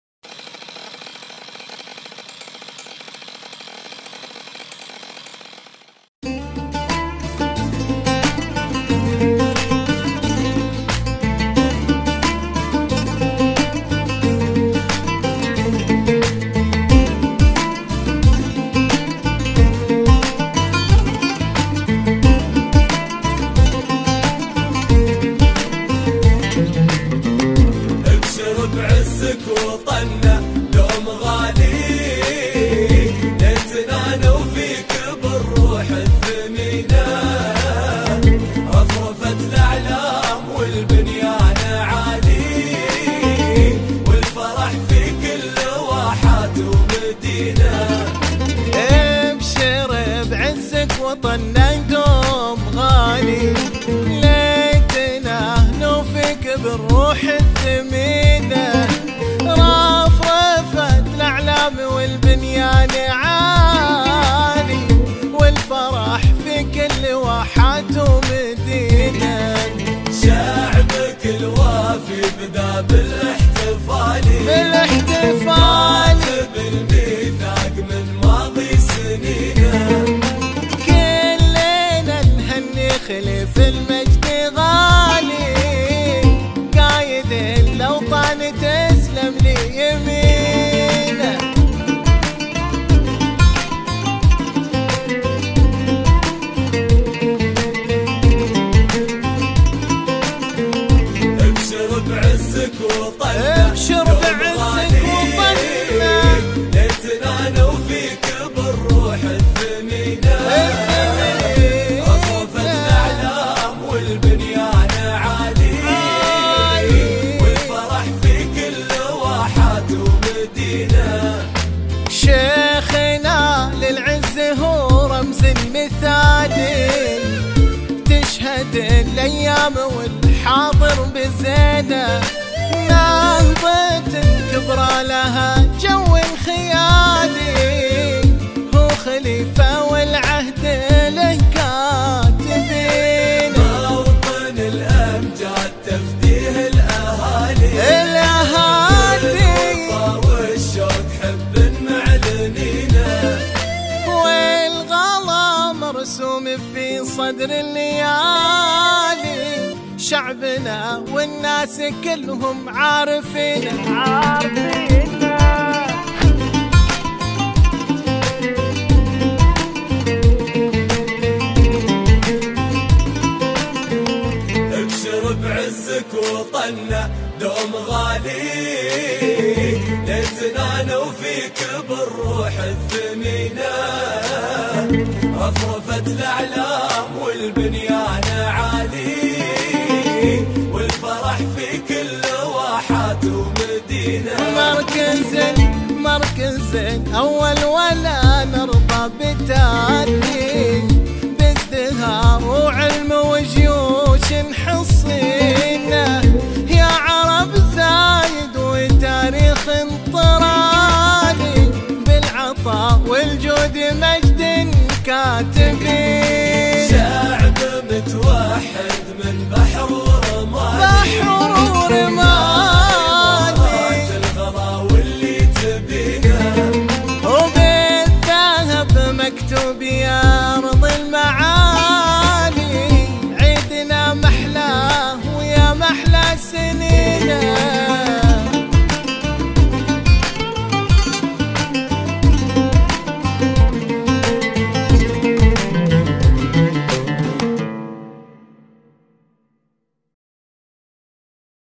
UAE National day
In the occasion of UAE's national day  I am glad to submit my heartfelt wishes UAE's  people many happy years to come